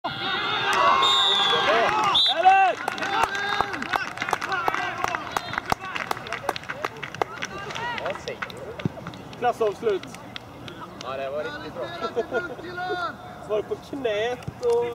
Kabel-tv: 3-1 mot Olsfors
Kabel-tv från Ryda sportfält i Borås där FC Kabel Åttio tar emot IF Olsfors i Boråssexan för herrar. De bruna brassarna vinner till slut matchen med 3-2. Klippet är från den andra halvleken, och Kabel har just gjort 3-1.
Målskytten är Christoffer Johansson på passning från Fredrik Enhörning.
Kommentatorerna i bakgrunden är Kalle Åkerström och Henrik Malm.